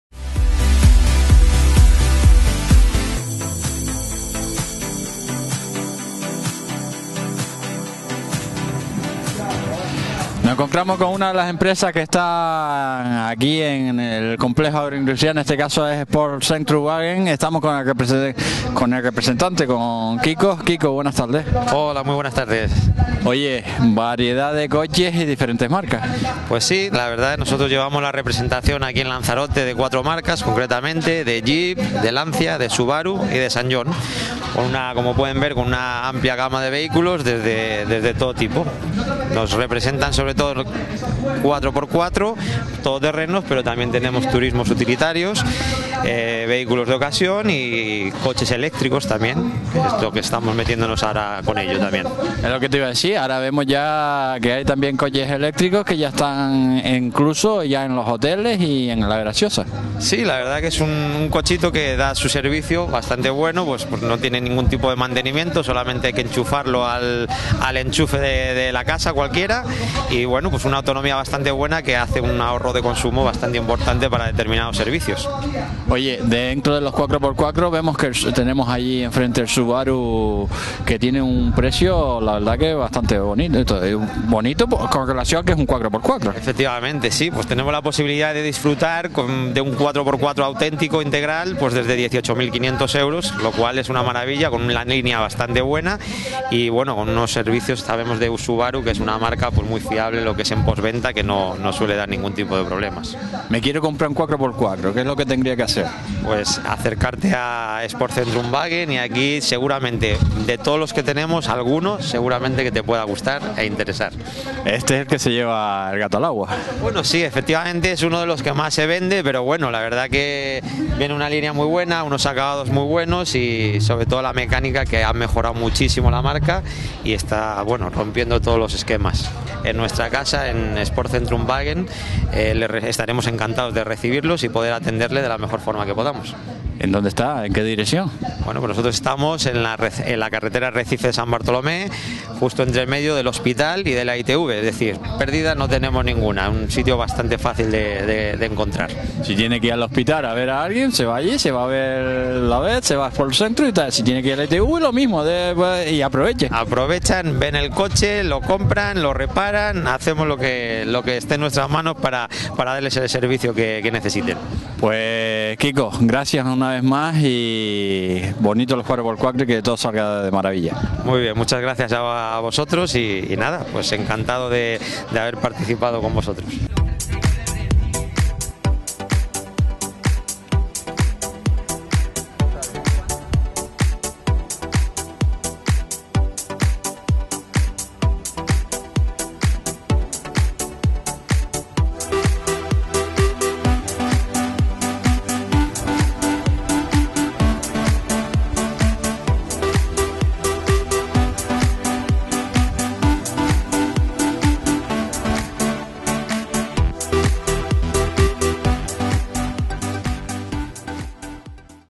SZWAGEN Expo
Exposición de Sport Zentrum Wagen en la Feria del Motor celebrada en Lanzarote con la gama de vehículos Jeep, Subaru, Ssanyong y Lancia.
SZWAGEN, concesionario de compra-venta de coches en Lanzarote.